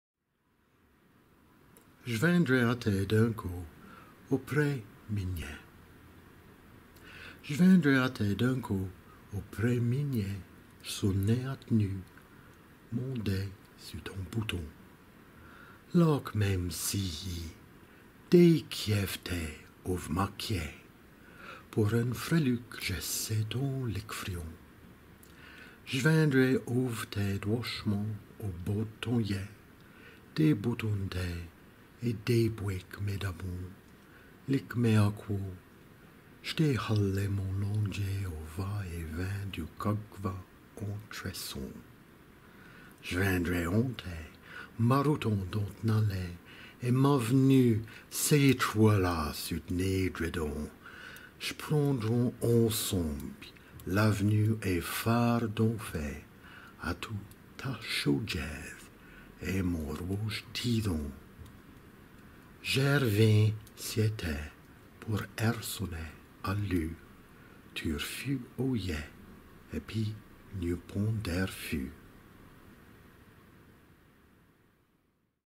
J'veindrai à té d'un co auprès mînniet
j'sonn'nai à t'n us, man dé sus tan bouton;
lâque-mé m'sîlyi, dêcliéve-té auve ma clié;
pouor eune fréluque jé s'sai tan liquefrion.

J'veindrai auve té douochement au bord d'tan liet;
dêboutonne-té et dêbouêque-mé d'abouon;
lique-mé acouo, j'dêhal'lai man landget
au va-et-veint du cagueva en tresson.

J'veindrai en té, m'arroutant dans t'n allée
et ma v'nue s'êtrueûl'la sus t'n êdrédon;
j'prendrons ensembl'ye l'av'nue ès feurs d'enfé;
atout ta chaûdgiéthe et man rouoge tîthon.

J'èrvîns siez té pouor èrsonner à l'us;
tu r'fus au liet épis n'y'eut pon d'èrfus.

Geraint Jennings